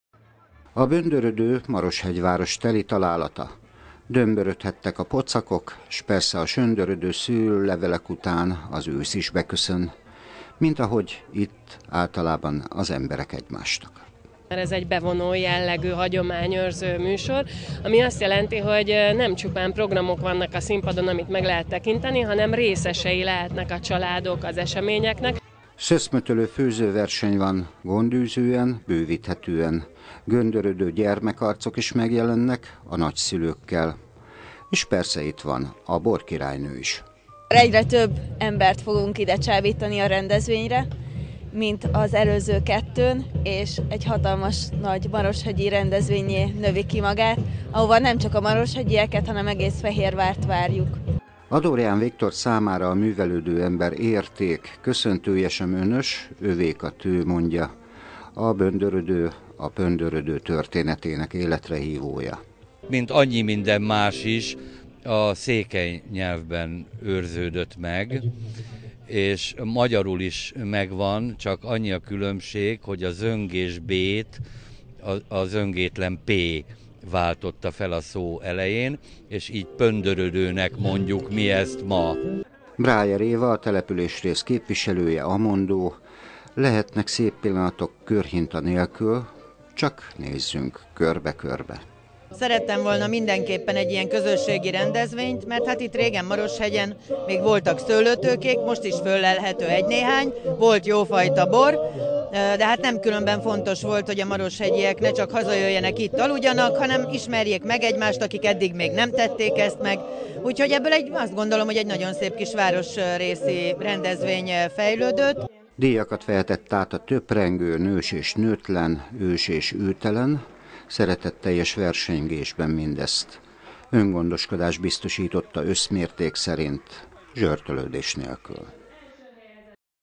Böndörödő Maroshegyen
Negyedszer rendeztek Maroshegyen szüreti mulatságot. A Böndörödőn felvonulás, hagyományőrző és szórakoztató programok várták a résztvevőket. A rendezvény keretében közel ötven csapat indult a főzőversenyen, a legjobbakat este, a rendezvény végén díjazták.